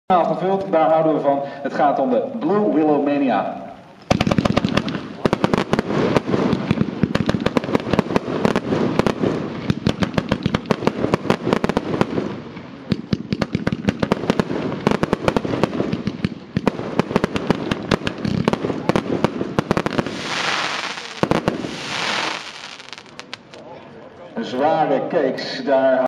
Blue Willow Mania Vuurwerkmania Geisha (nieuw 2018)
Grootste kaliber en maximaal gevuld. Wat een gigantisch dikke bak is dit. Willow time! Uit de PyroRebels lijn. Art.nr. ZFC 1728. Dit filmpje is geschoten tijdens de regenachtige demo-avond van Vuurwerkmania op 29 augustus 2018 bij Flevonice in Biddinghuizen. Er worden er 2 naast elkaar geschoten maar ik heb de camera op het linkerexemplaar gericht.